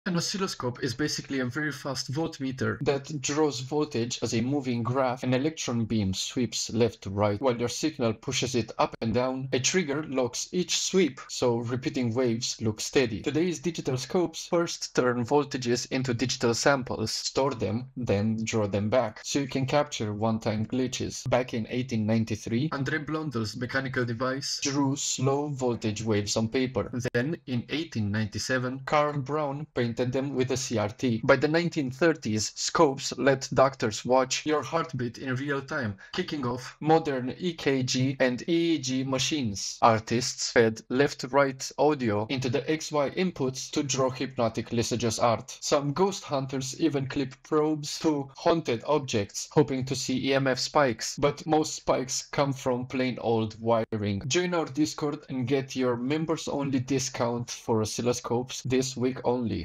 From glowing CRT traces to digital scope art, we break down every waveform
Har du nogensinde spekuleret på, hvordan den glødende grønne kurve kan afsløre hjerteslag, male Lissajous-kunst eller endda opfange spøgelsesagtige EMF-pigge? I dag dykker vi ned i, hvordan oscilloskoper forvandler rå spænding til betagende visuelle oplevelser – fra Blondels første papirgraf til moderne digitale tricks. Tænd din nysgerrighed, del dine egne scope-hacks, og slut dig til vores Discord-fællesskab for flere nørdede snakke og tips! (link i bio)